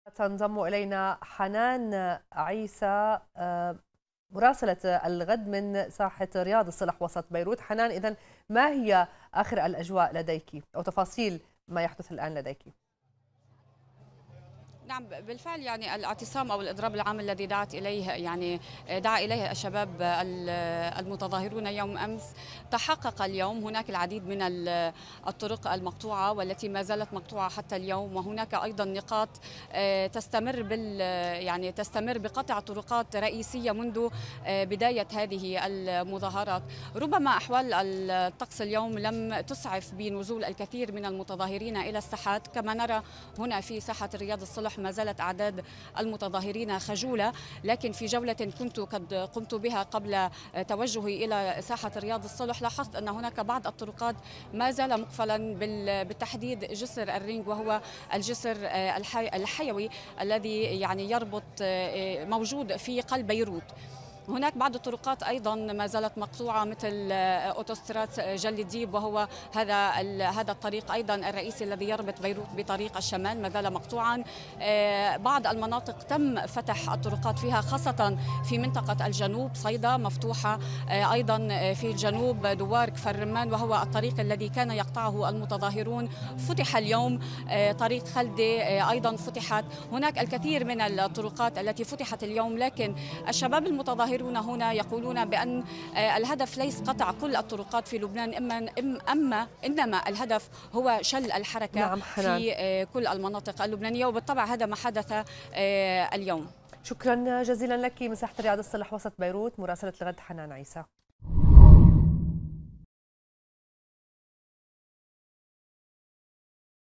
استمرار إغلاق الطرق بالسيارات في لبنان مع دعوات للإضراب
دعا المتظاهرون في لبنان إلى تنظيم إضراب عام اليوم في جميع أنحاء البلاد , وغلق الطرق بالسيارات .
يأتي هذا في الوقت الذي تتواصل فيه الاحتجاجات لليوم الثاني عشر في العاصمة بيروت، للتأكيد على مطالب رحيل كل الطبقة الحاكمة، وأفادت مراسلة الغد بأن هناك العديد من الطرق المقطوعة في أنحاء متفرقة من البلاد بدءًا من بيروت ومحيطها وصولا إلى الجنوب.
المزيد من التفاصيل سنعرفها من مراسلة الغد من ساحة رياض الصلح في وسط بيروت حنان عيسى مع الإعلامية يارا حمدوش عبر النشرة الإخبارية.